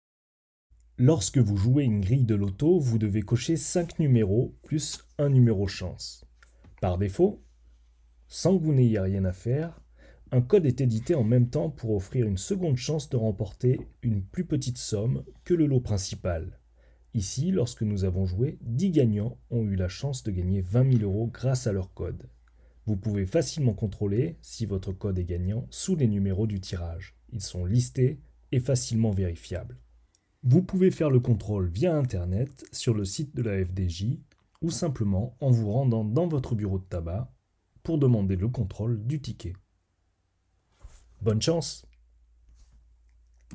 🏆 COMMENT VÉRIFIER SI SON CODE LOTO EST GAGNANT ?
Lorsque vous jouer une grille de loto, un CODE LOTO est édité en même temps et vous donne une seconde chance de remporter une somme complémentaire.

Comment le vérifier ? C'est très simple, vous allez le voir dans la vidéo :)